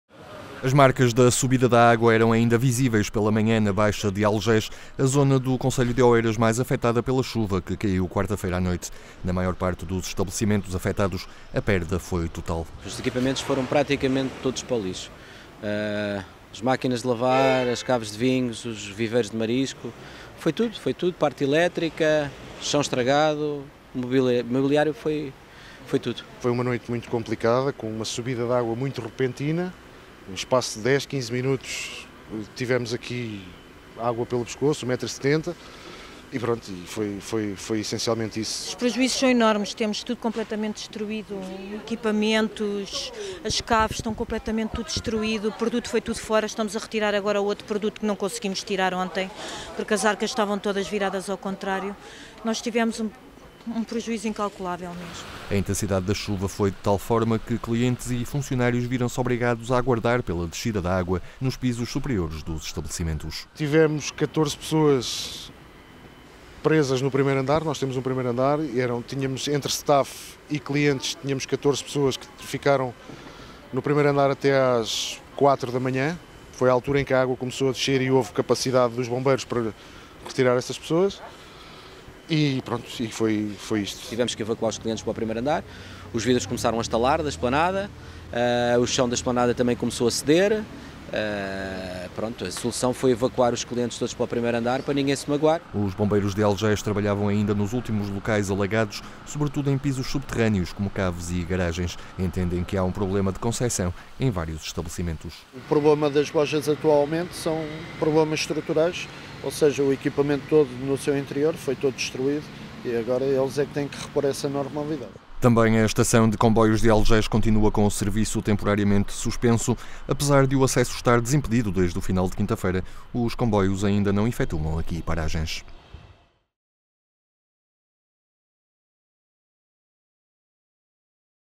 Marcas da subida de água ainda visíveis na baixa de Algés
As marcas da subida da água ainda são visíveis na baixa de Algés, a zona do concelho de Oeiras mais afetada pela chuva que caiu na quarta-feira à noite.